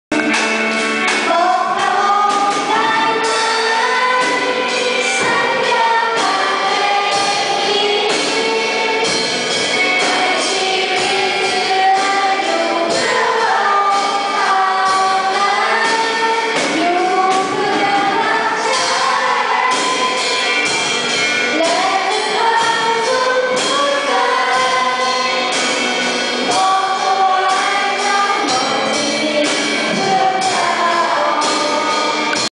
Church Plant Visit - Sept 20, 2015
Troy spoke at church and we spent time with the kids afterwards. God is doing amazing things here!